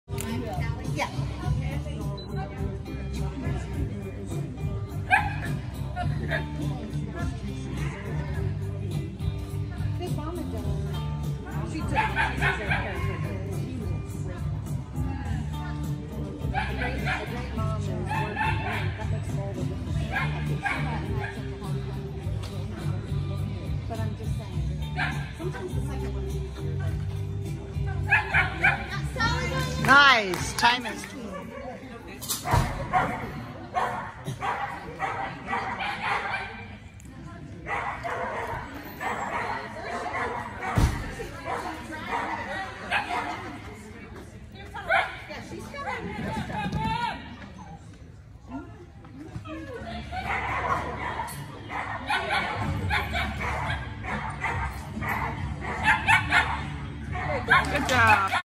Remedy QQ 7 and 8
Only runs I filmed, but Remedy got two QQBB this weekend—here are her jumpers runs.